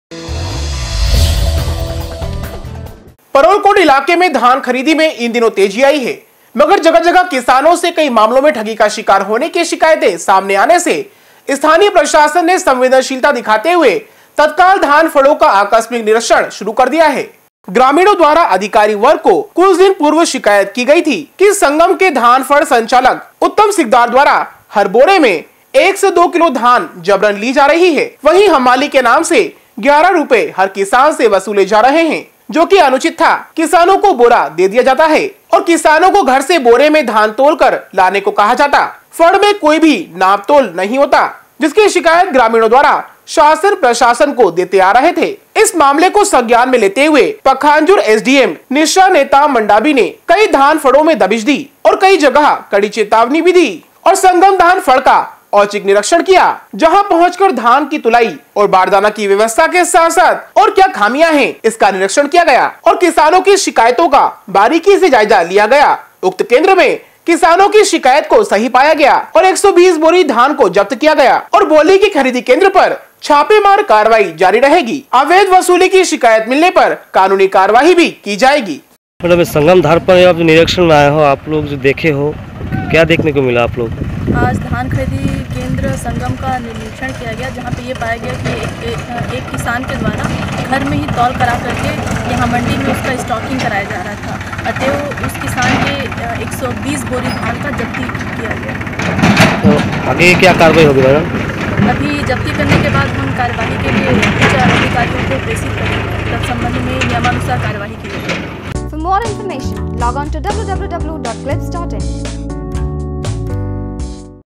Exclusive: धान फड़ संचालक की मनमानी से किसान परेशान
परोलकोट इलाके में धान खरीदी में इन दिनों तेजी आई है मगर जगह जगह किसानों से कई मामलों में ठगी का शिकार होनें की  शिकायत सामने आने से
स्थानीय प्रशासन ने संवेदनशीलता दिखाते हुए तत्काल धान फड़ो का आकस्मिक निरीक्षण शुरू कर दिया है।